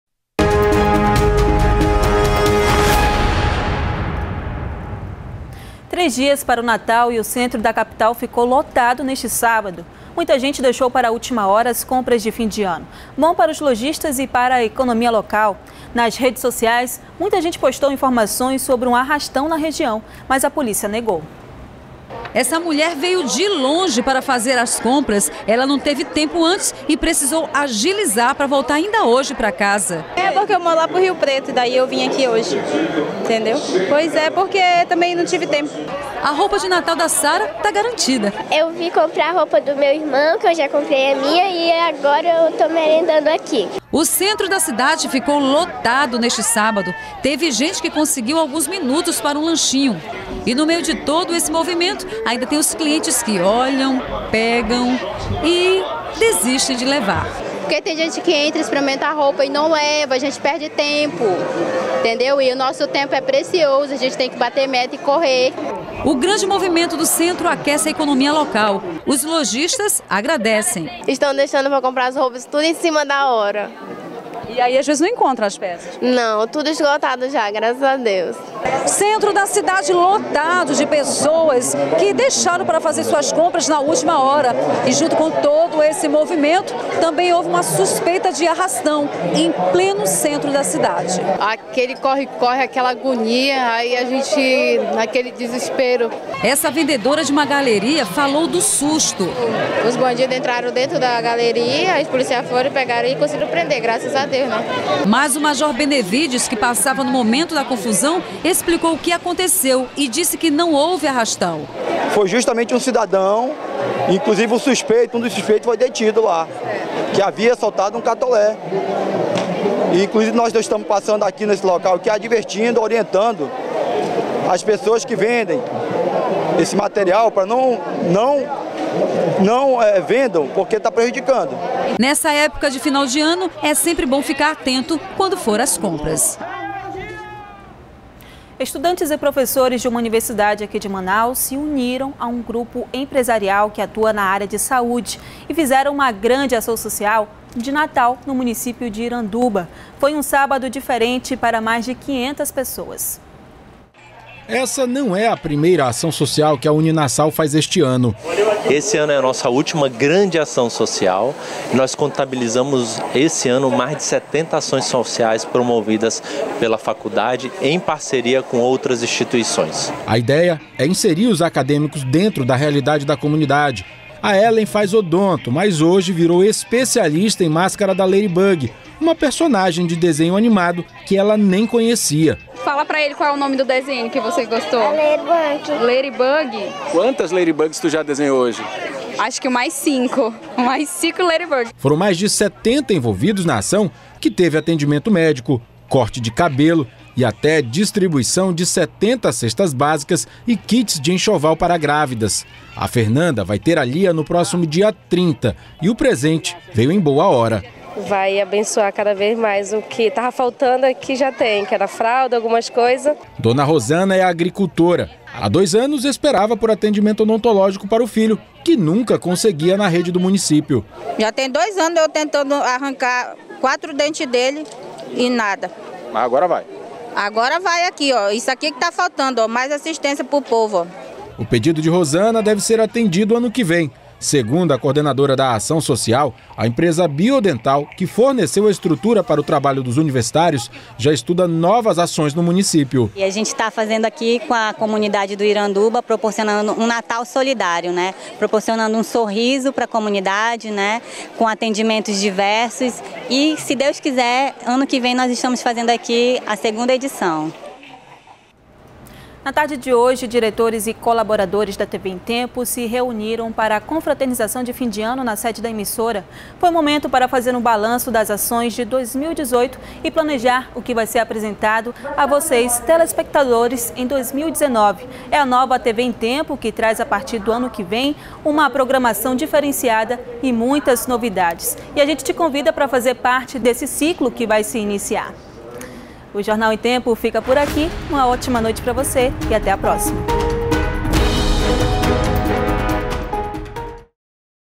Iranduba: Estudantes e professores de uma universidade fizeram uma grande ação social de natal
03º BLOCO JORNAL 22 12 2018 - Iranduba: Estudantes e professores de uma universidade fizeram uma grande ação social de natal